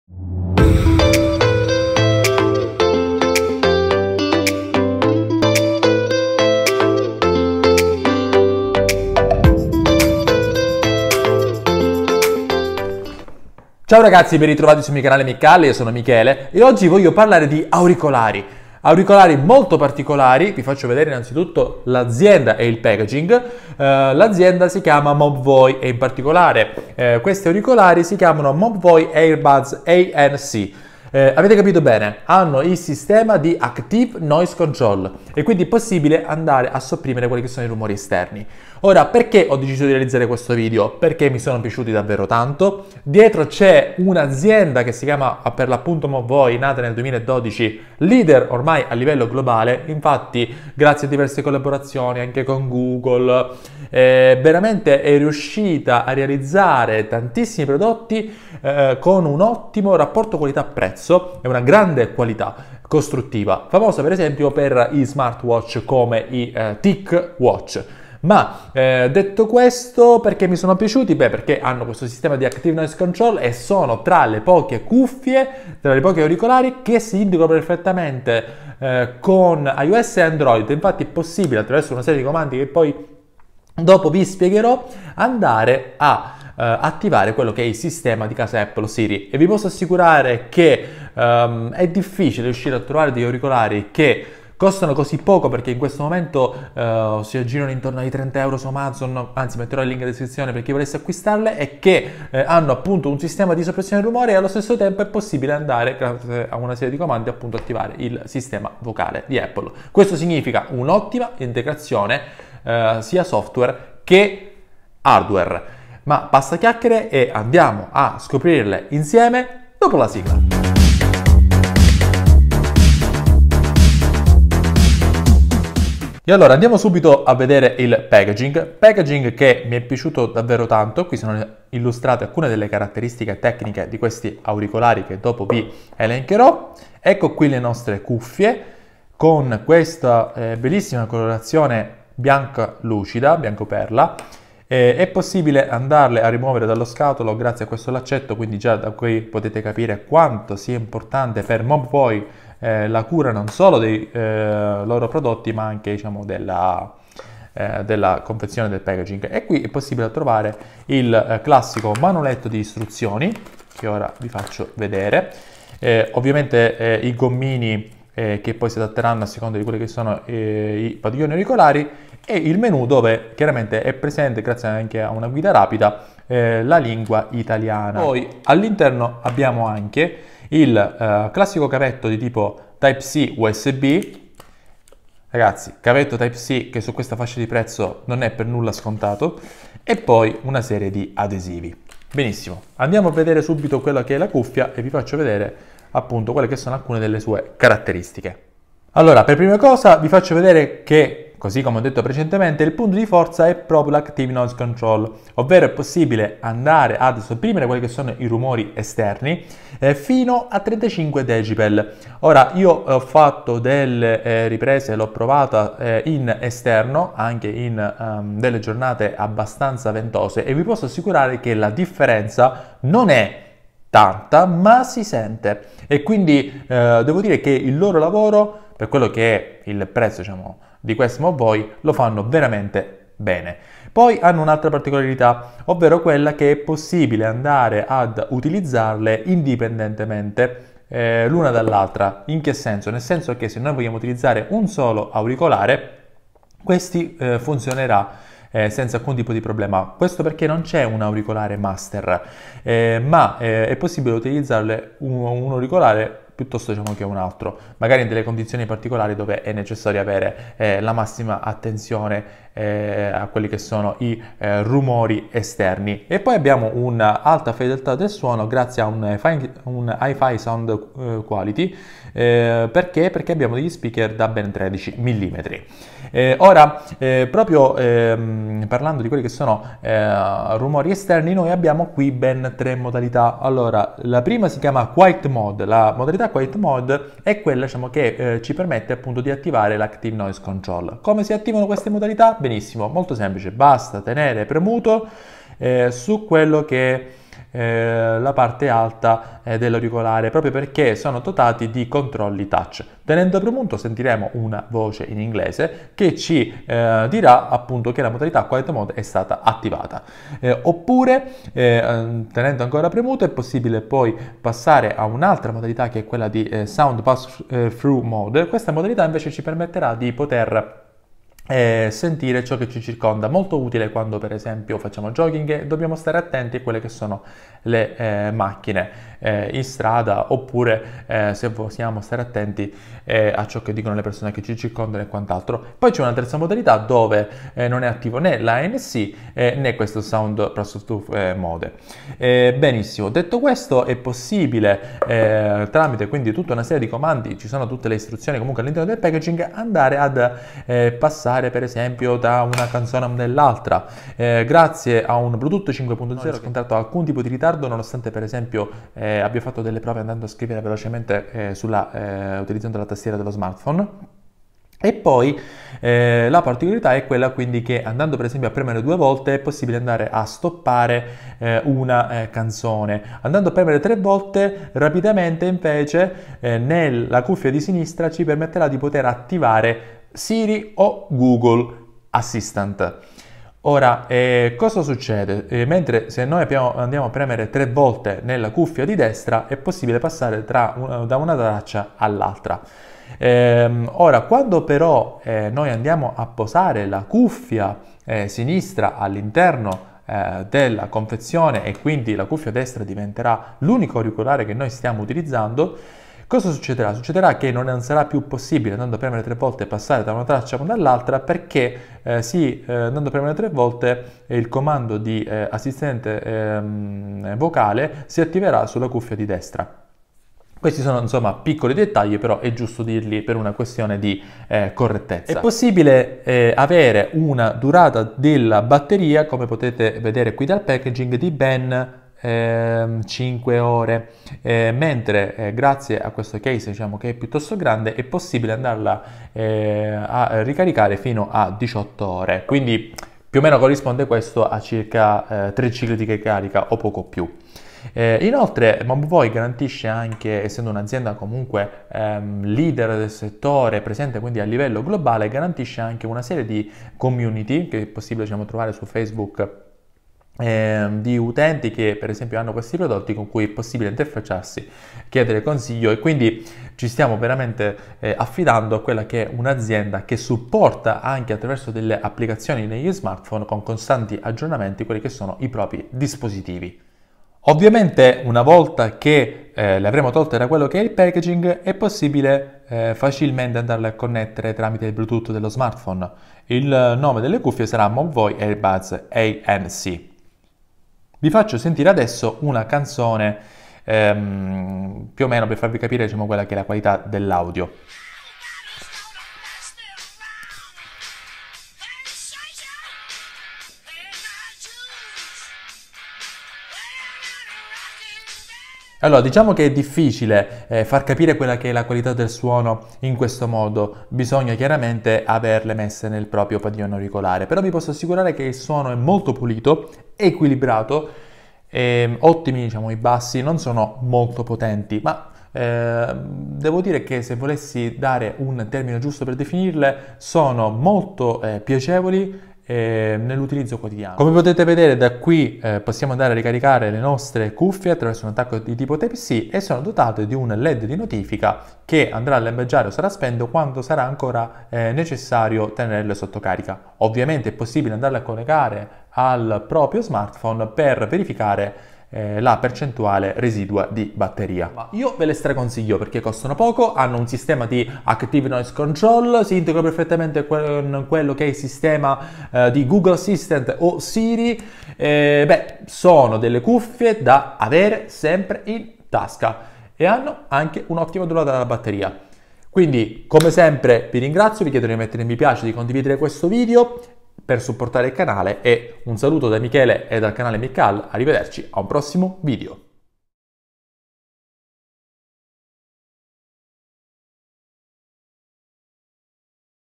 Recensione Mobvoi Ticpods auricolari Earbuds ANC con soppressione attiva del rumore a soli 29 € !!
Music intro by

Musicista: @iksonmusic

Link di acquisto:

TicPods ANC Bluetooth 5.0 con cancellazione attiva del rumore, cuffie resistenti all'acqua IPX5 con auricolari wireless, cuffie touch control

Il contenuto di questo video ha esclusivamente finalità di informare.